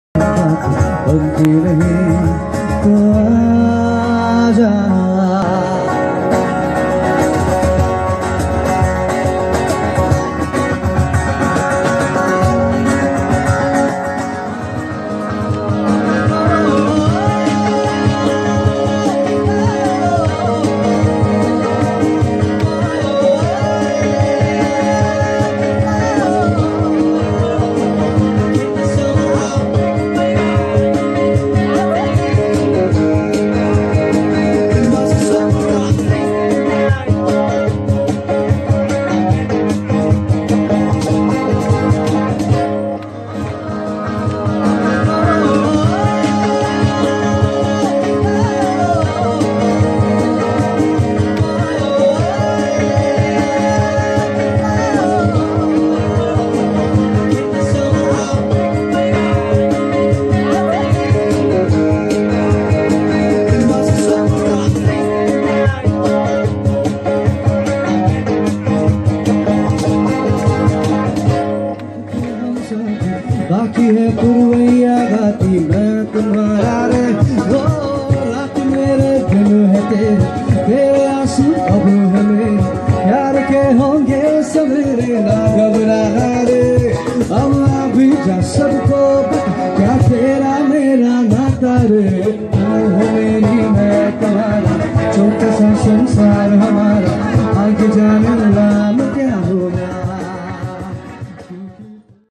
Urban Monkz live at What's Up Cafe
The Urban Monkz had a great time performing at wonderful venue of What's Up Cafe. There's nothing better than an upbeat crowd, enthusiastic hosts and good music.